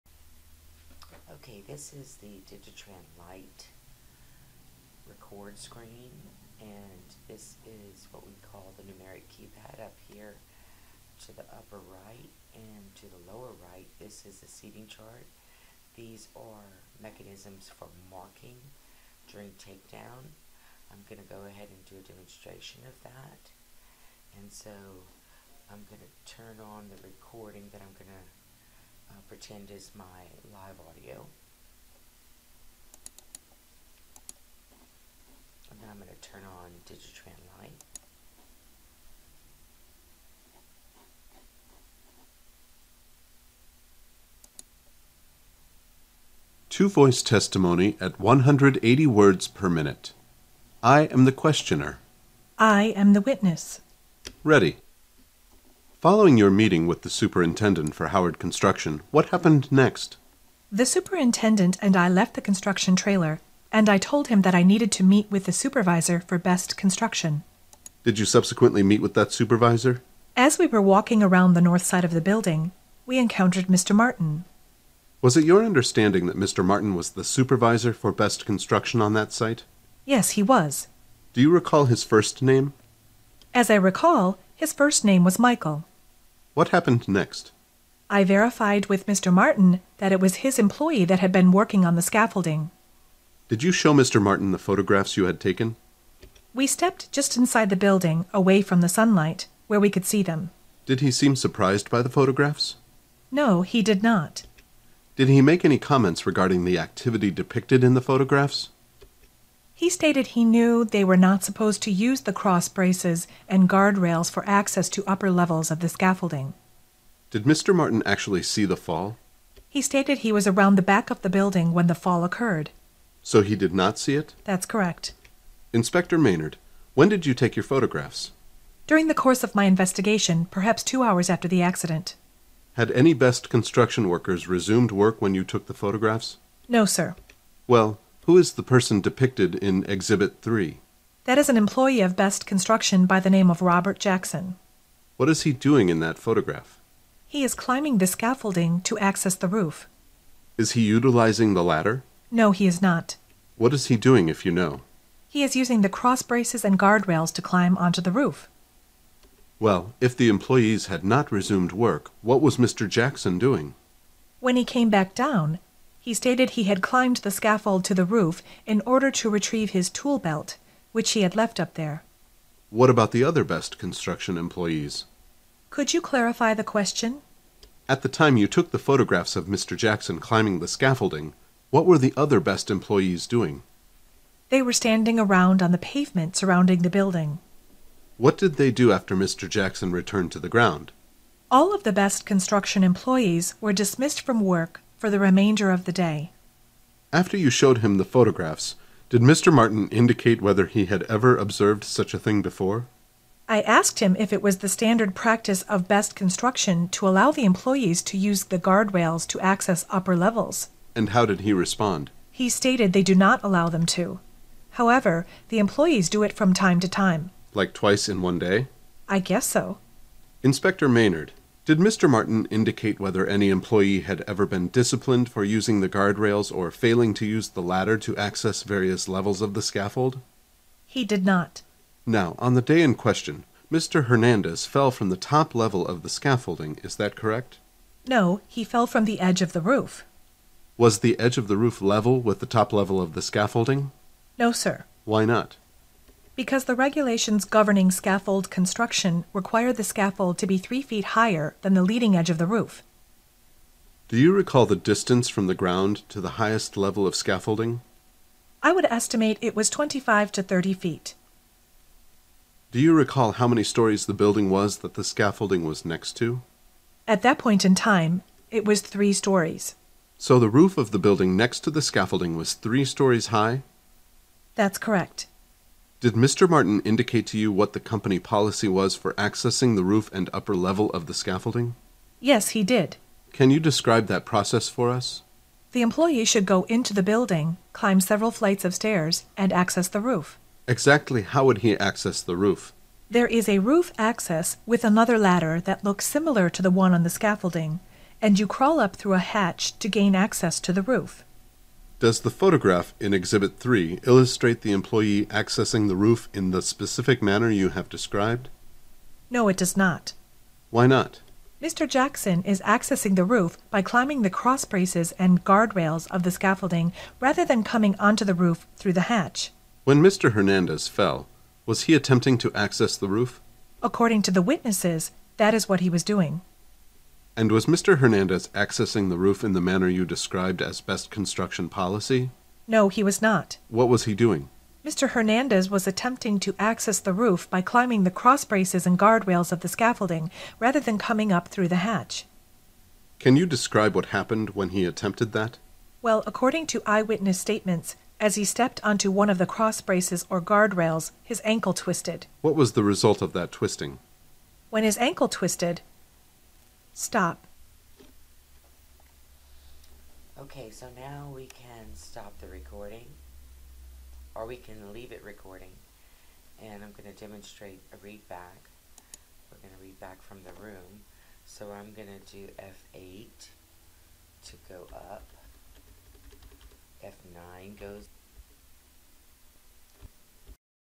DigiTran Lite Demo
This is a Demonstration of DigiTran Lite which also displays the forms of Marking that are available with that application.